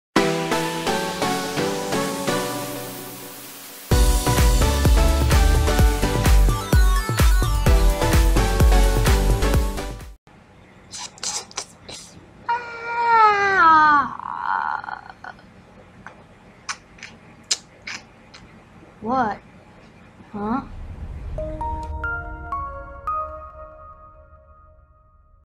Shrinking Potion(minecraft animation)
I use Mine-imator
Shrinking Potion !!!
Edit 2 years later: Why does this get so many views i think its real cringy